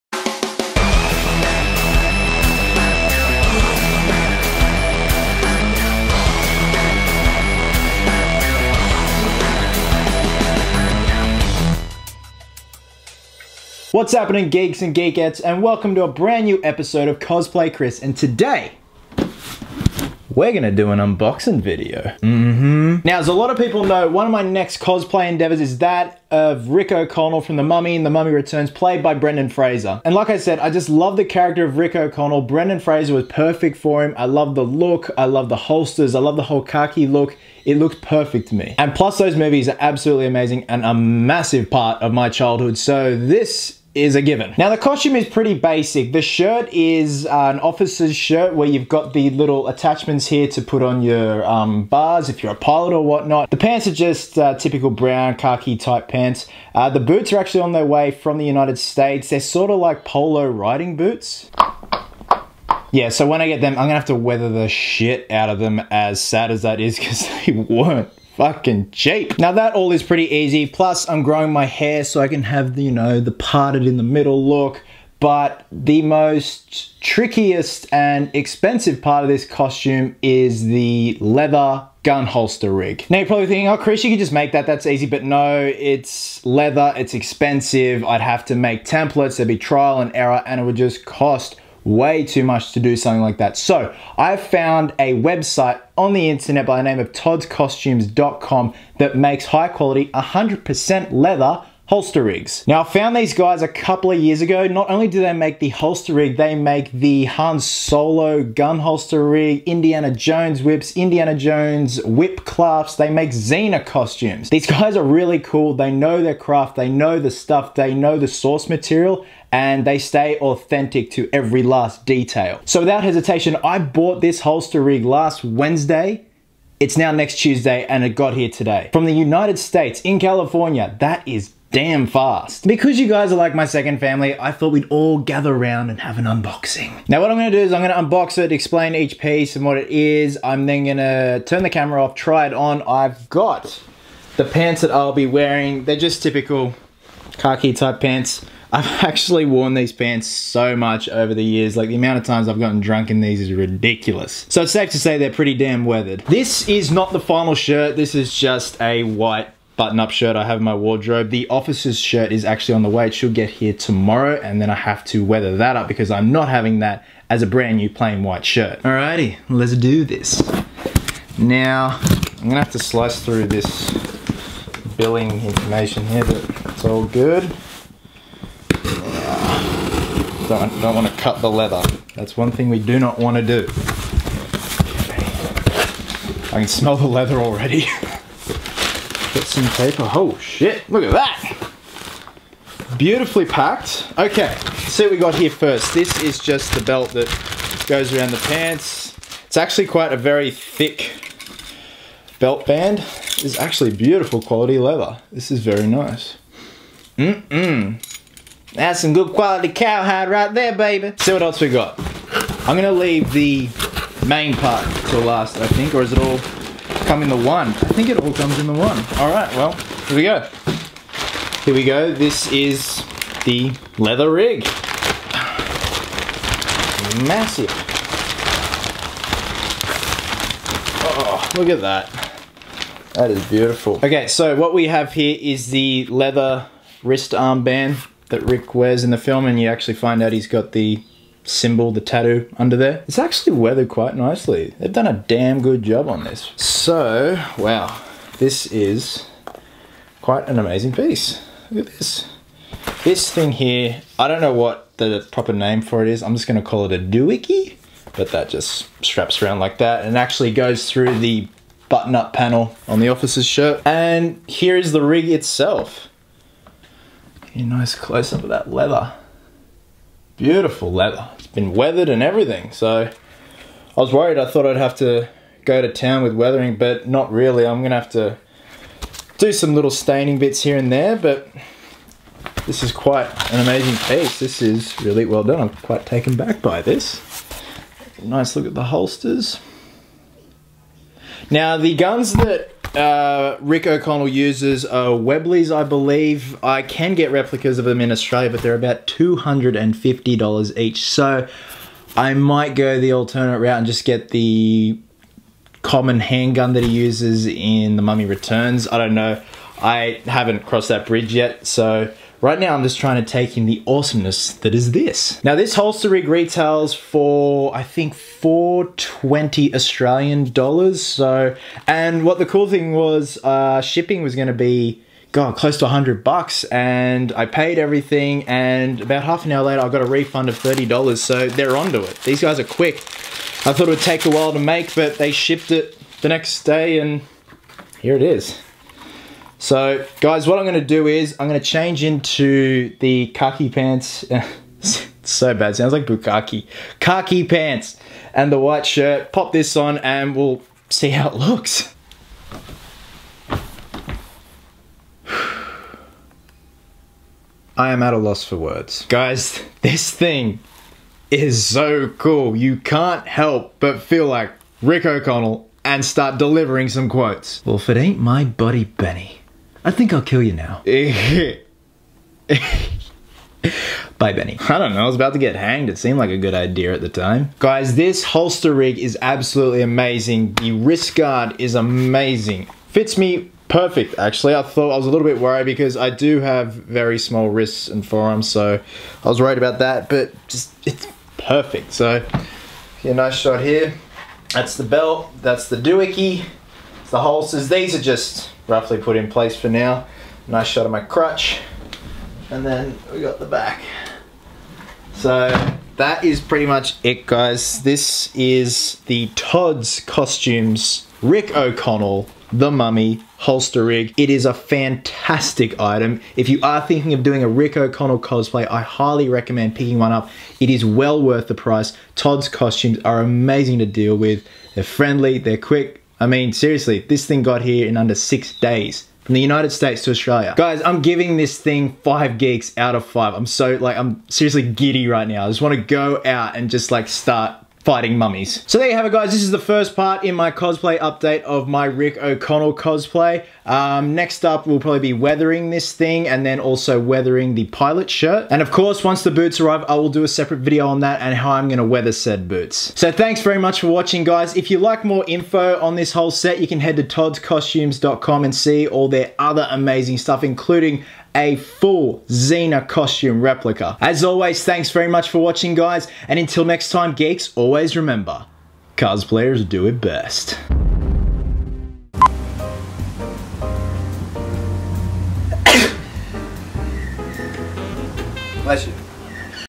The Mummy: Rick O'Connell Cosplay Update- Gun Holster Rig
Well Geeks, as promised here is the first part in a series of updates on my Rick O'Connell Cosplay! First up, an amazing leather gun holster rig unboxing. The piece was made by the very talented